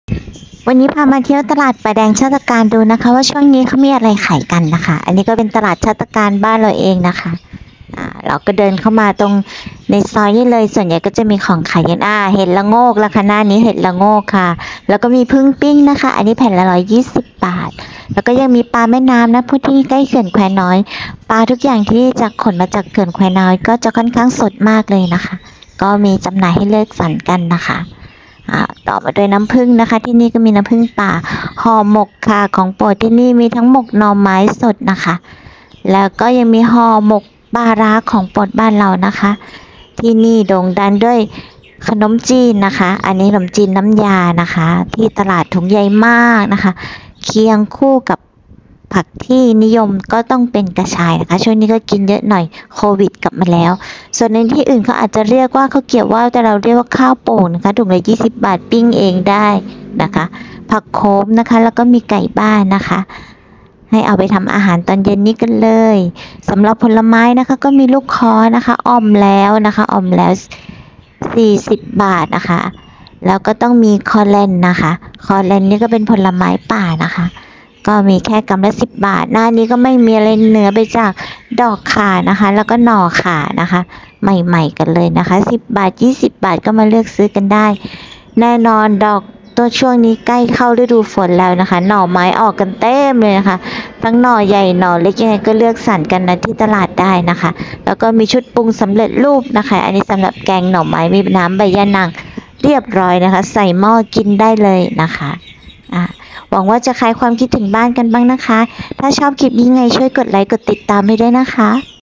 ตลาดป่าแดง ชาติตระการ มีอะไรขายบ้าง ย่างเข้าหน้าฝน
ตลาดป่าแดง อยู่ที่ ต.ป่าแดง อ.ชาติตระการ
มีของป่า อาหารพื้นบ้านให้เลือกมากมาย แวะมาจับจ่ายใช้สอยกันค่ะ